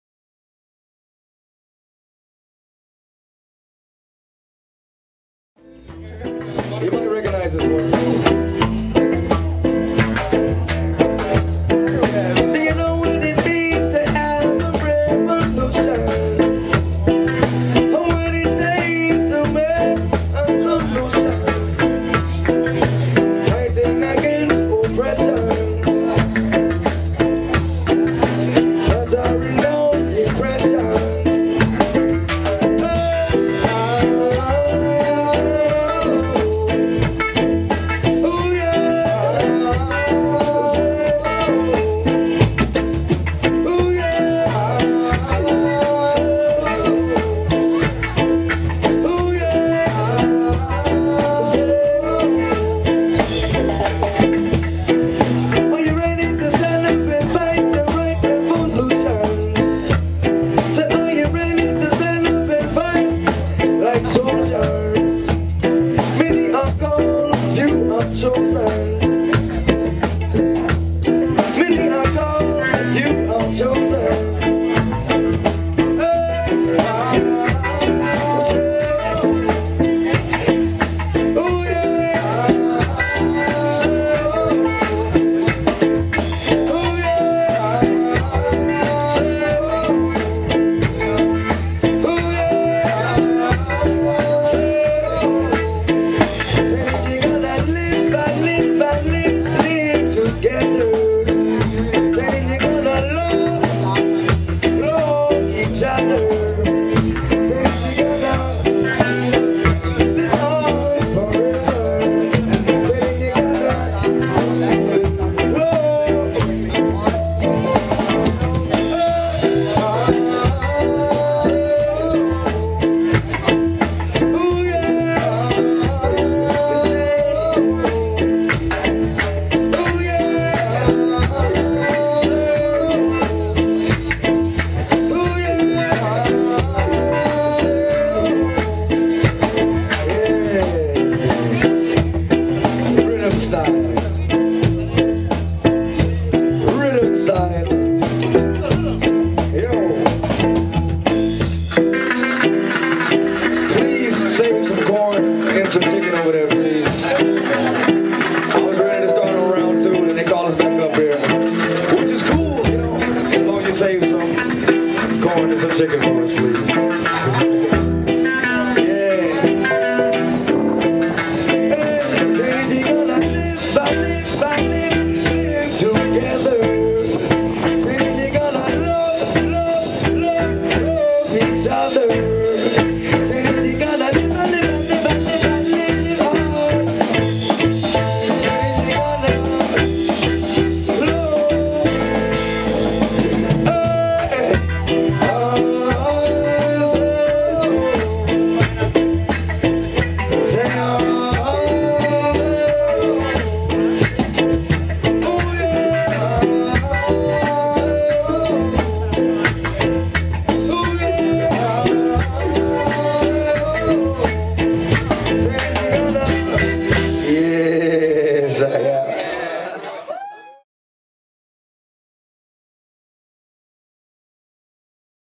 Arise Roots 2
Arise Roots coming live and direct from Los Hot Boxers Head Quarters.